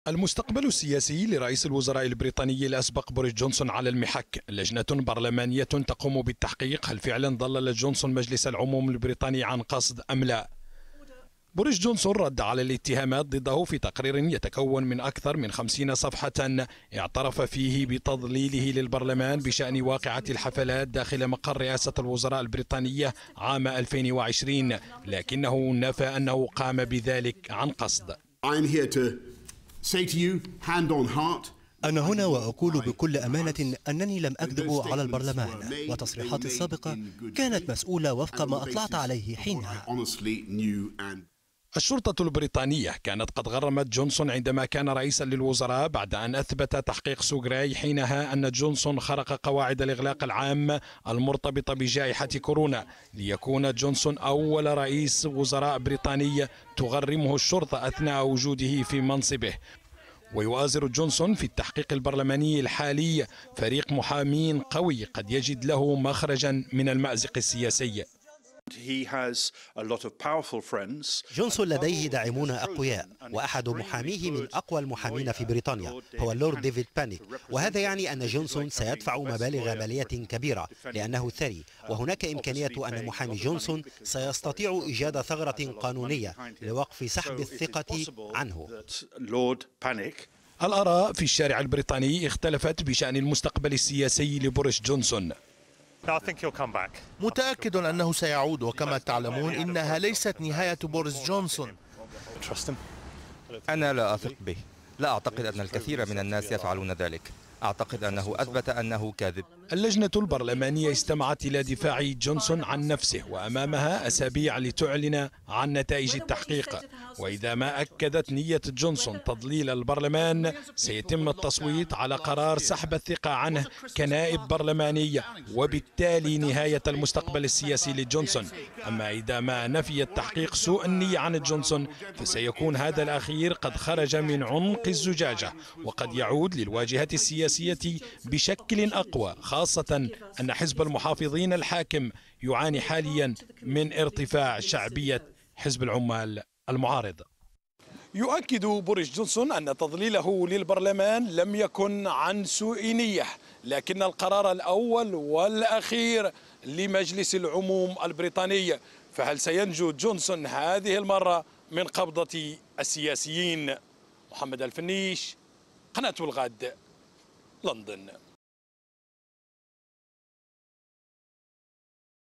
بريطانيا | تحقيق برلماني في اتهام جونسون بتضليل العدالة في قضية الحفلات
يواجه#رئيس_الوزراء_البريطاني_الأسبق بوريس جونسون تحقيقا برلمانيا بشأن هل فعلا ضلل جونسون البرلمان عن قصد أم لا وذلك على خلفية حفلات تمت في مقر رئاسة الوزراء البريطانية في نهاية عام ٢٠٢٠ خلال فترة الإغلاق العام إبان جائحة كورونا عندما كان جونسون رئيسا للوزراء... المزيد من التفاصيل عبر التقرير المصور لمراسل الغد من لندن محمد الفينيش، عبر الفقرة الإخبارية.